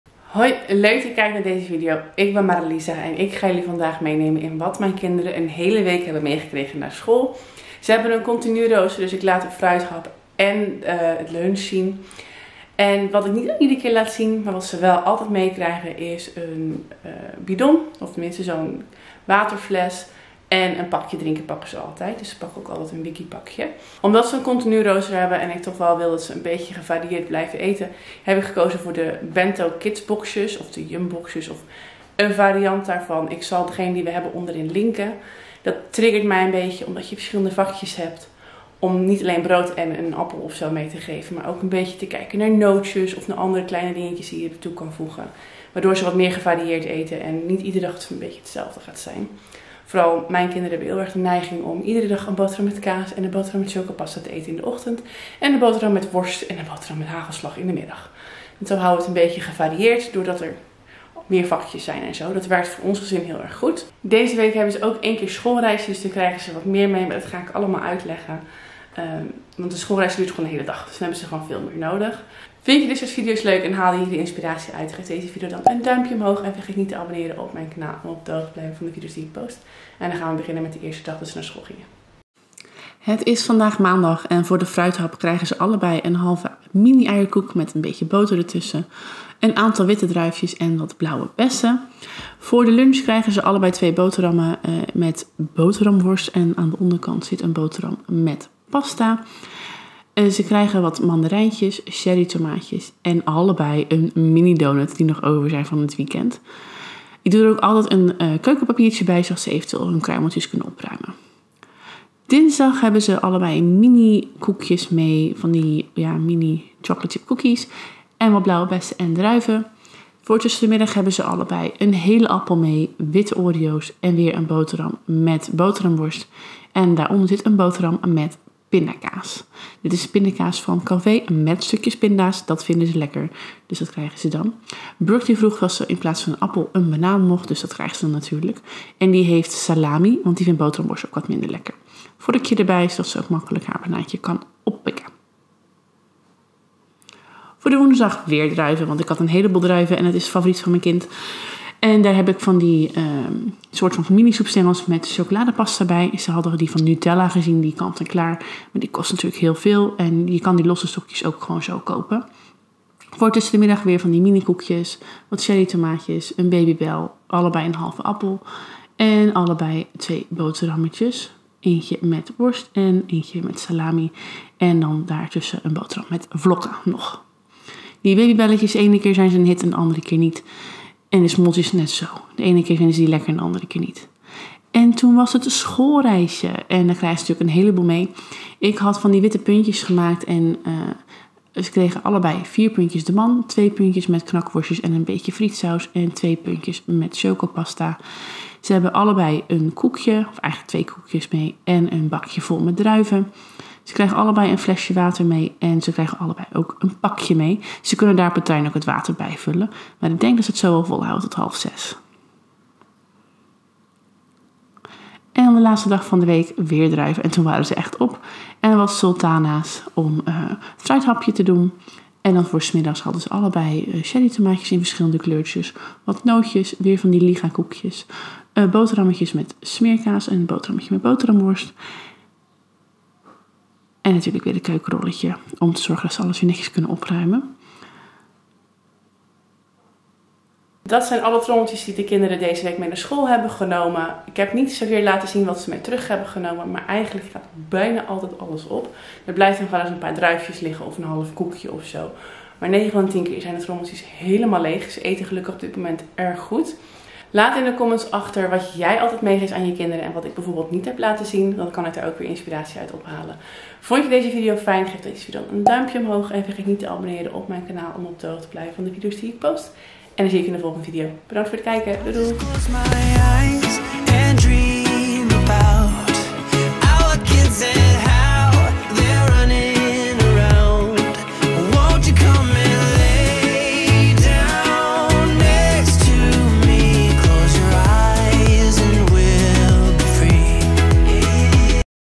WAT KRIJGEN ZE MEE NAAR SCHOOL | Mariliza Pauwels
Logan en Brooke hebben een continu rooster wat betekend dat ze door de weeks op school lunchen, Ik probeer hier en daar wat variatie toe te brengen in de maaltijden om er voor te zorgen dat ze ook gemotiveerd zijn om hun trommeltjes leeg te eten.

Persoonlijk vind ik de bento style lunch en fruit doosjes ideaal omdat ik daarmee ook uitgedaagd wordt om verschillende dingen in hun trommeltjes te doen. Logan en Brooke hebben deze: